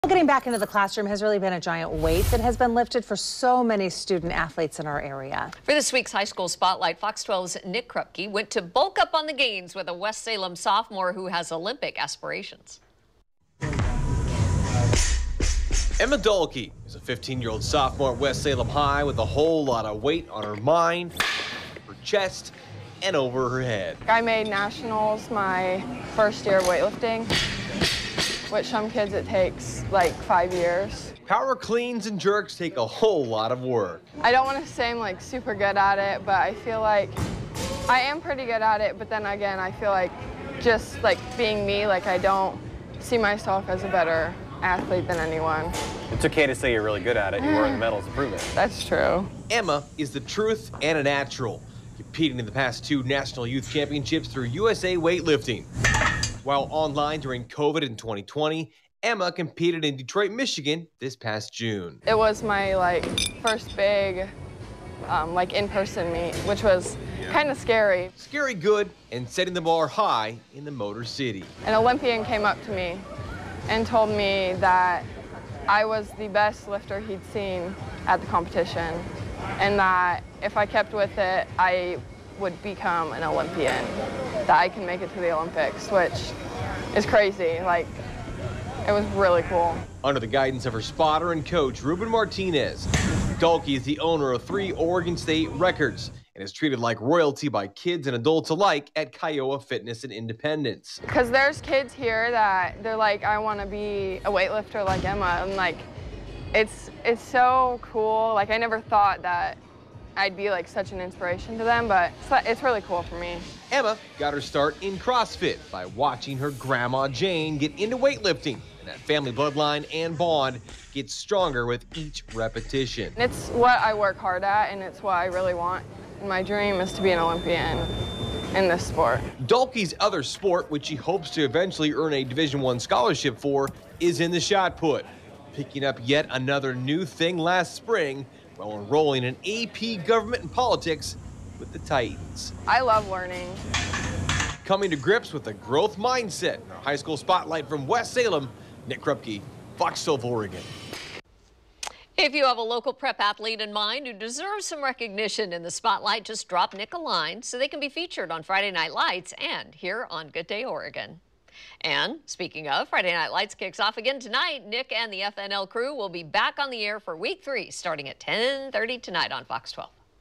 West Salem High sophomore raising the bar for Olympic dream
Emma Dalke is a 15-year-old sophomore at West Salem High School with a whole lot of weight on her mind, her chest, and over her head.